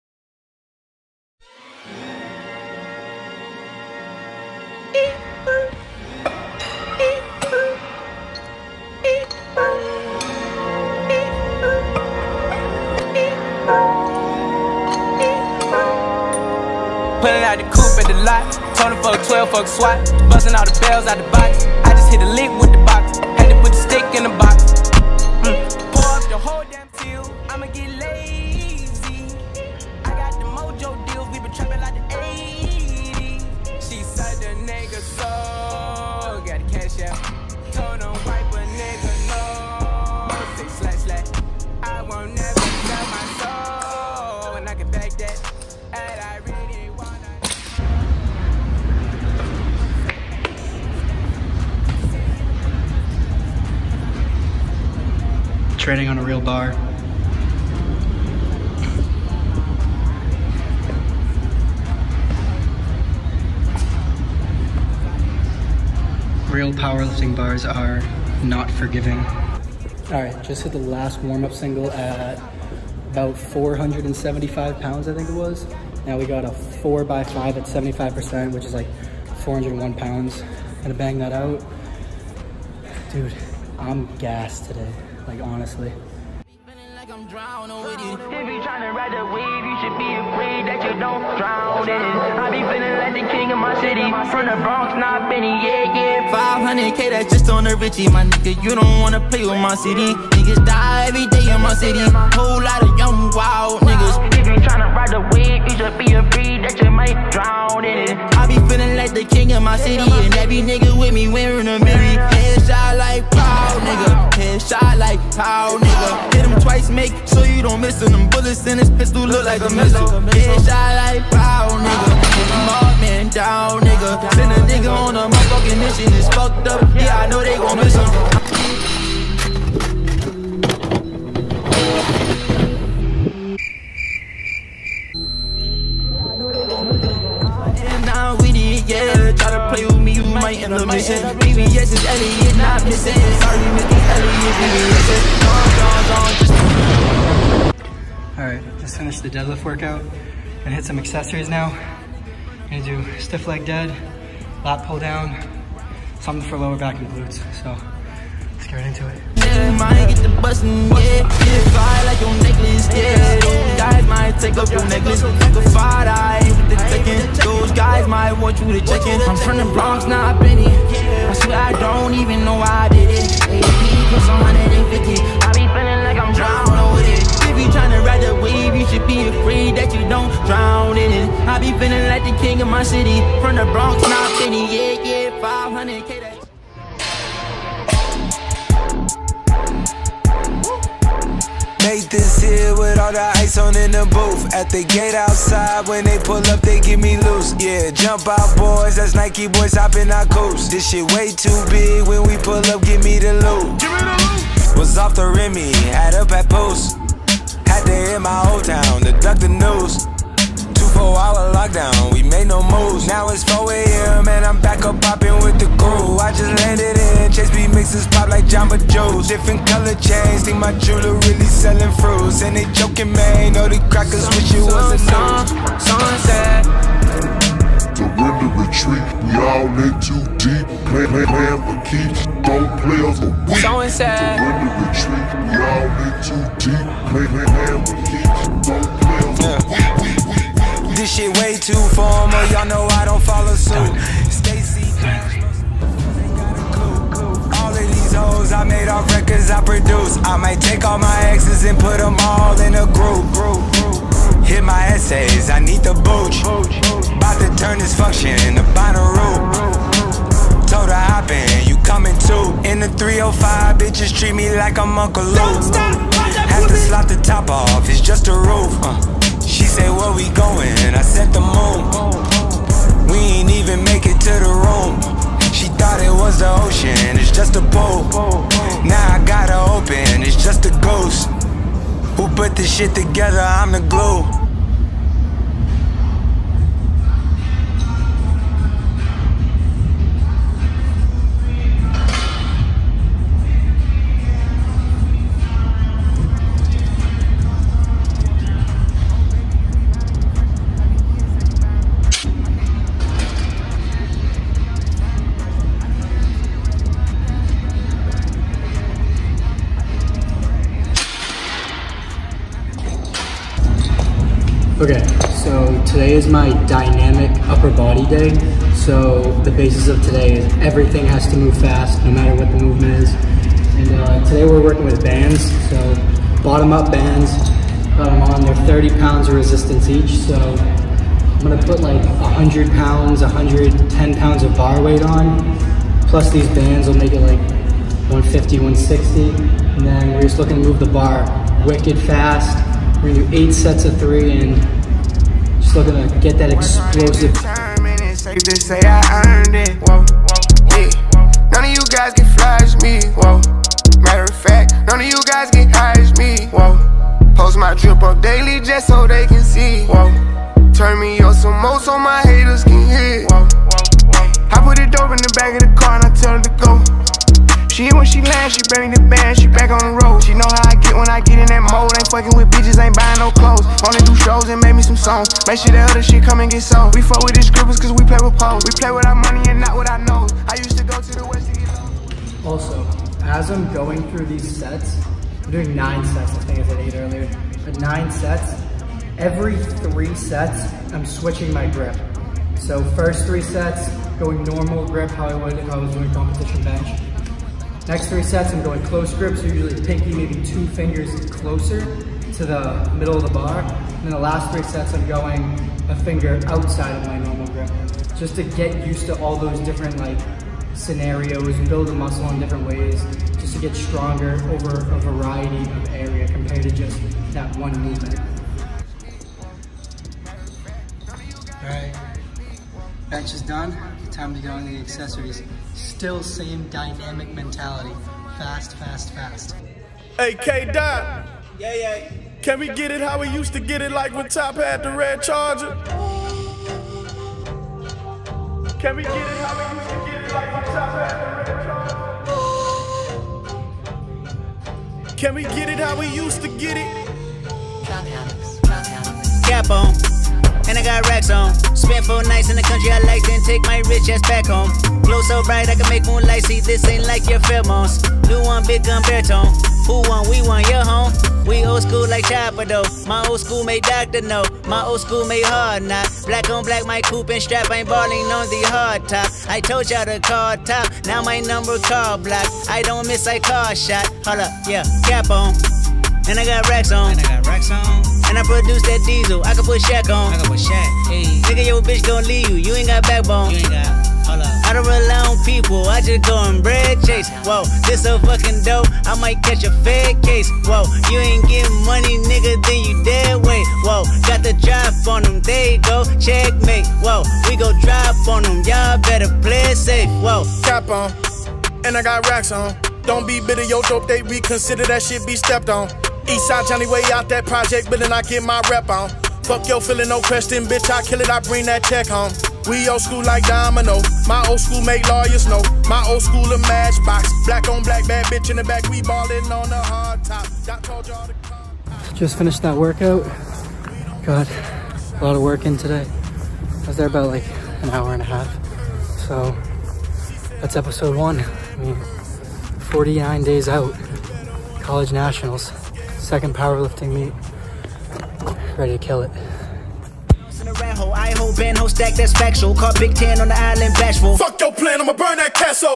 The Prep Files College Nationals: Episode 1
Episode 1 of my College Nationals Prep series. This episode showcases a deadlift workout and a dynamic bench workout.